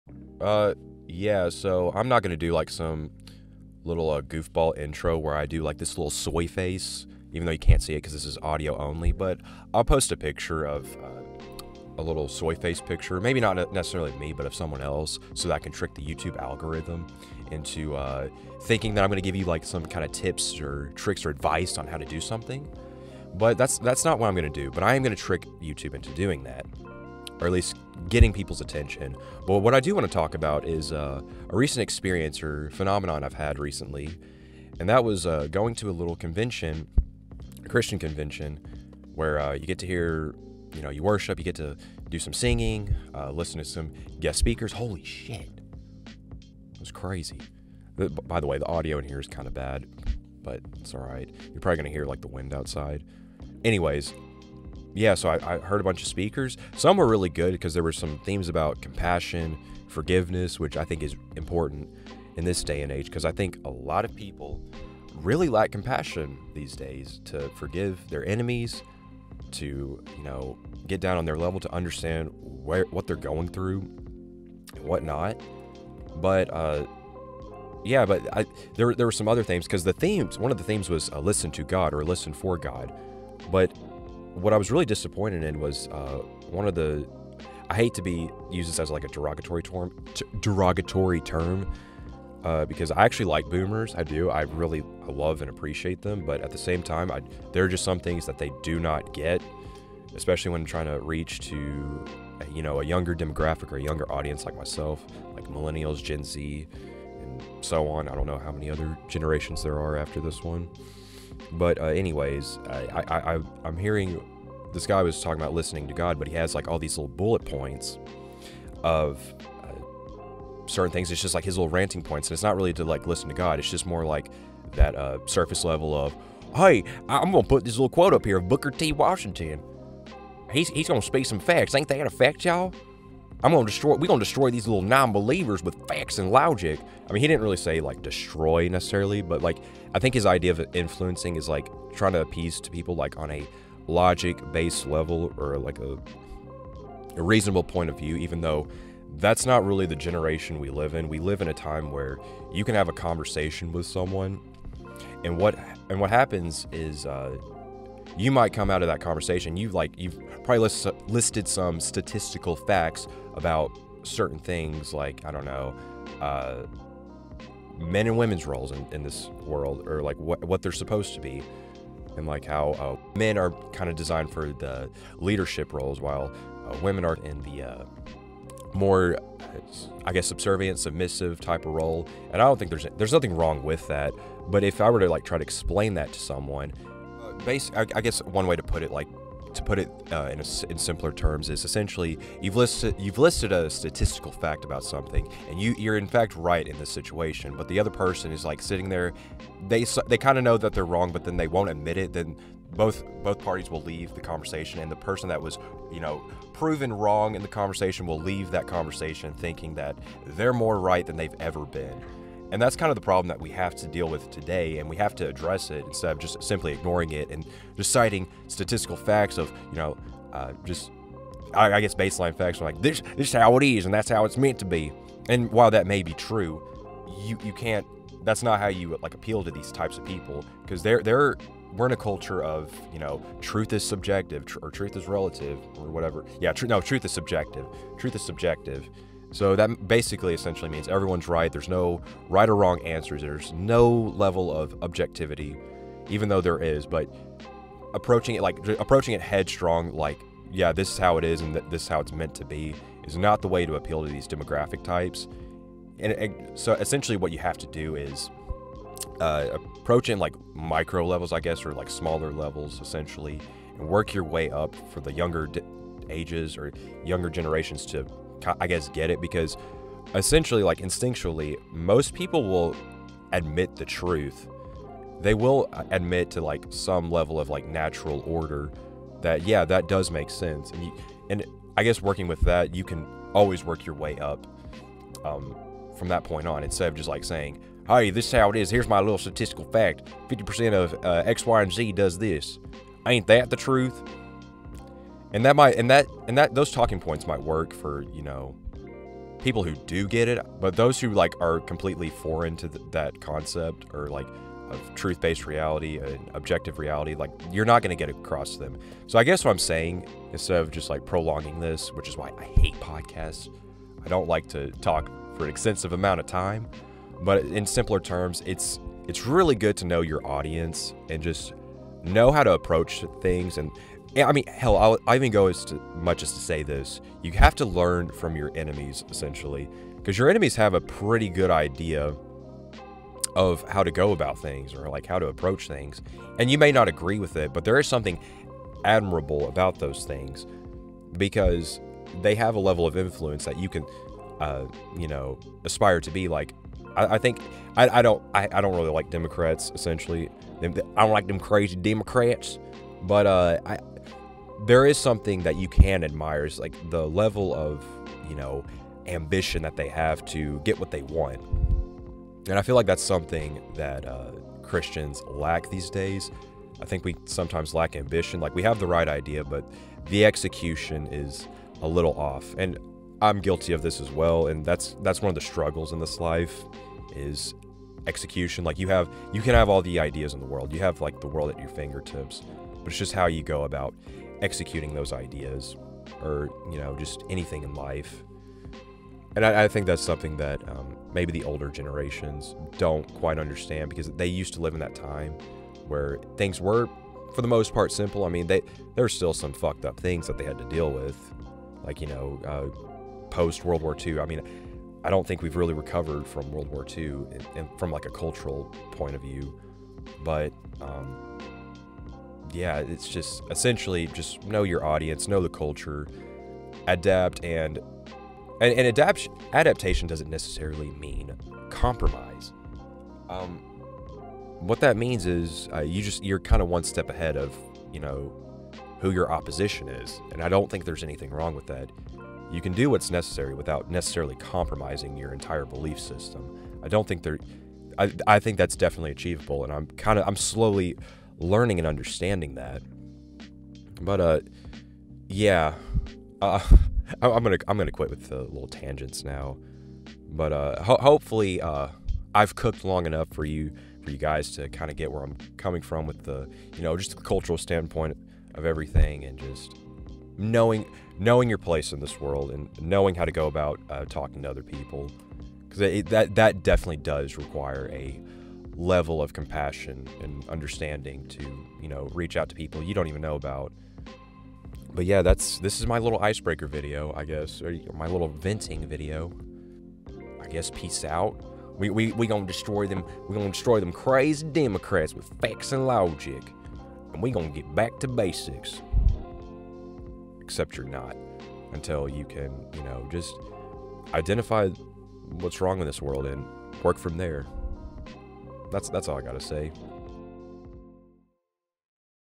5 *CRAZY* WAYS TO OWN THEM DEMOCRATS (YOU WON'T BELIEVE WHAT HAPPENS NEXT)
WELCOME TO THE SQUISTOL POD, WHERE WE TALK ABOUT NOTHING IN PARTICULAR UNTIL WE DO (OR ME IN THIS CASE). ENJOY MY FIRST LACKLUSTER EPISODE 1: THEM CRAZY DEMOCRATS

CREDIT TO THE CHANNEL ELI ELI FOR THE BACKGROUND LOOP